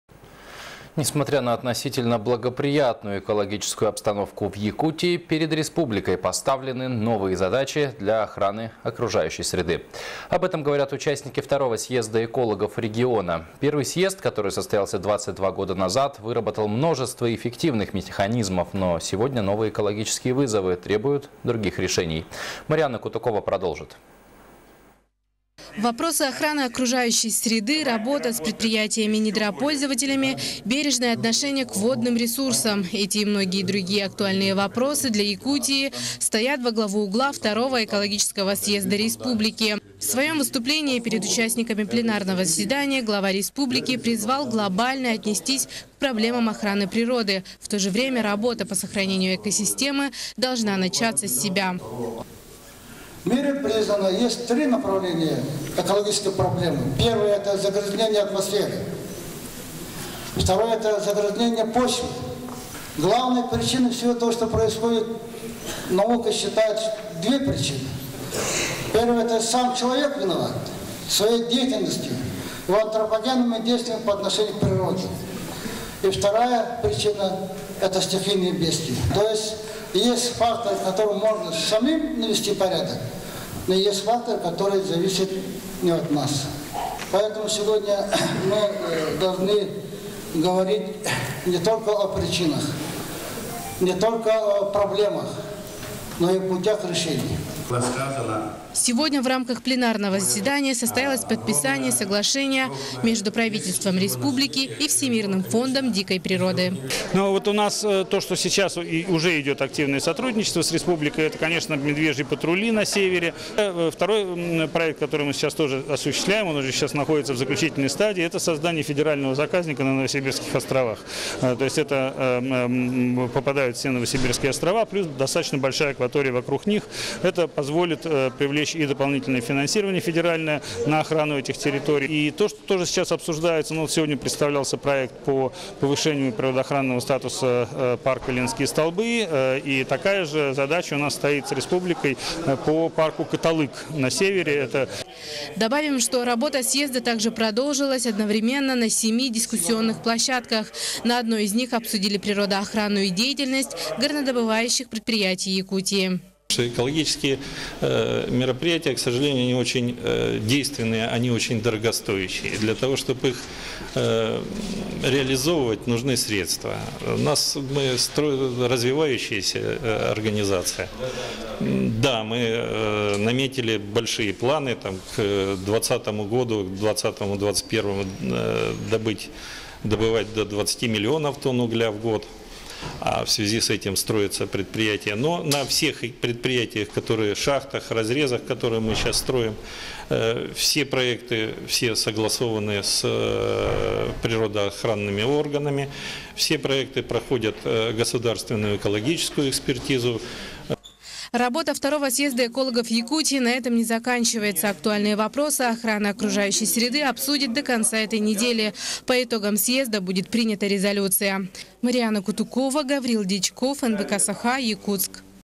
Новые задачи по охране природы. В Якутске проходит 2-й съезд экологов республики
Новые задачи по охране природы. В Якутске проходит 2-й съезд экологов Якутии